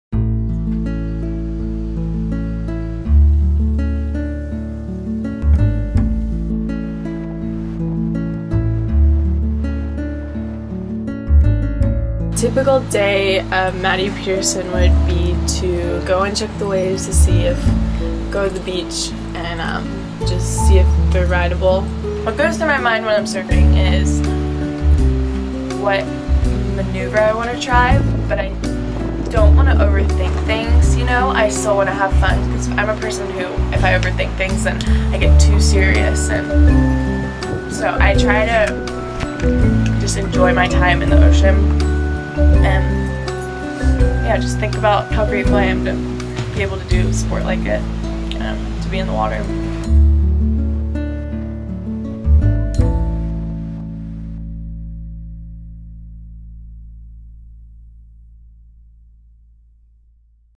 Surfing: A Model Reflects - Photography Studio, Philadelphia PA
A Light Illusions Studios short.

Maddie Peterson shares her thoughts and feelings on why surfing is important in her life.